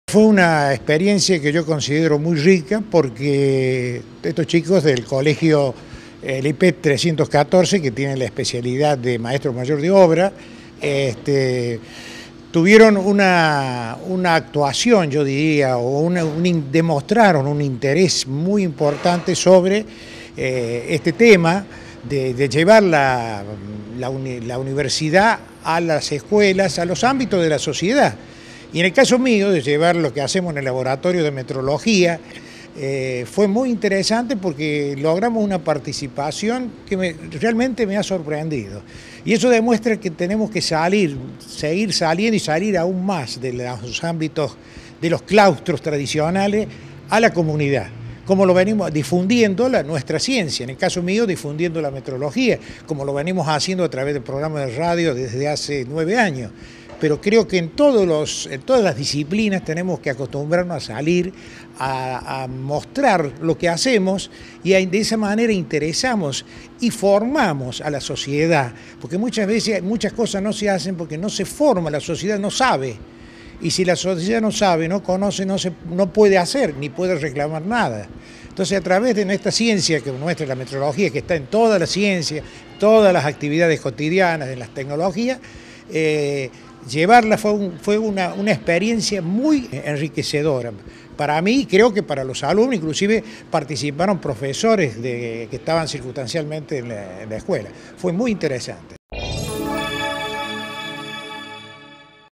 Continua el ciclo café científico: Gustavo Zocco
GUSTAVO ZOCCO - DOCENTE DE INGENIERÍA. PRESENTARON LA CHARLA “METRO LOGÍA ¿POR QUE Y PARA QUE?”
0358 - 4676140